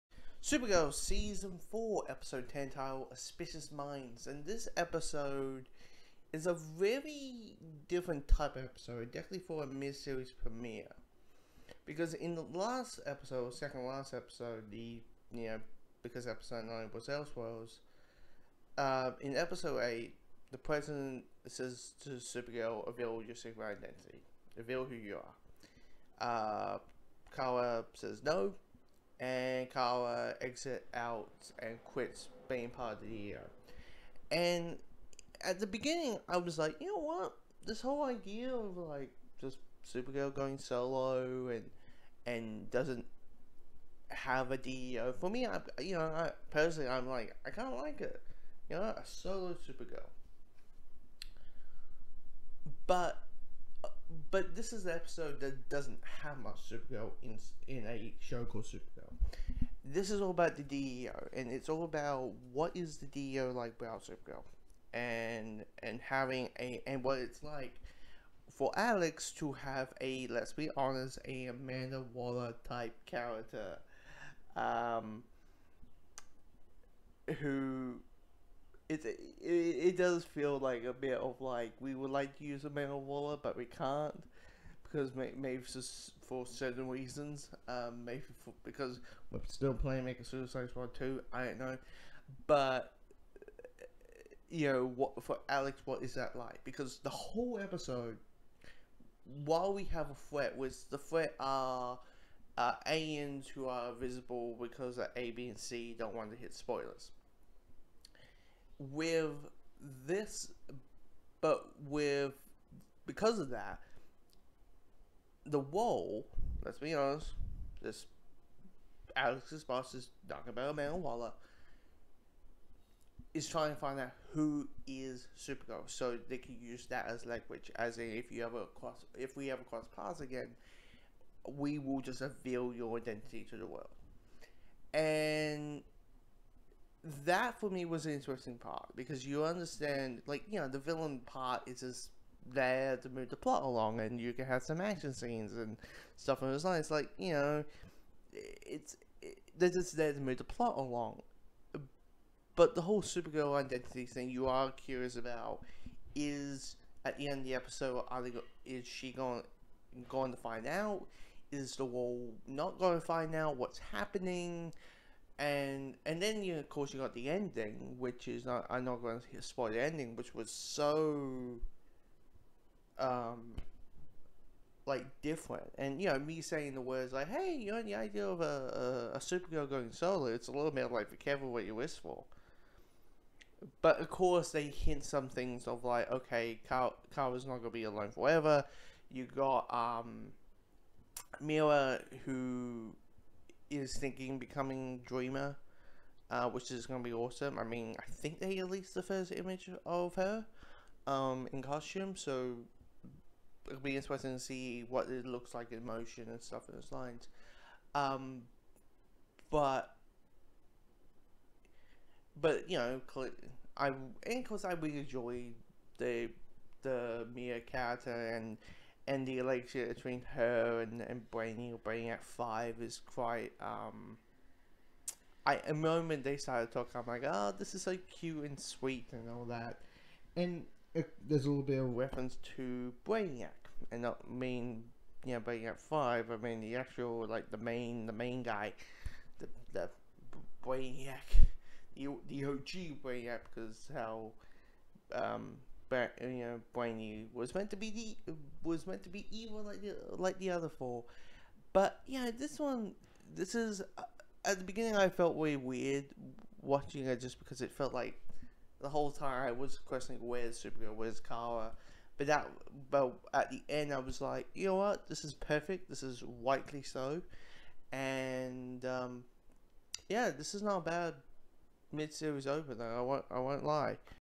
Supergirl: Suspicious Minds - Season 4 Episode 10 Review
I review the season 4 mid season premiere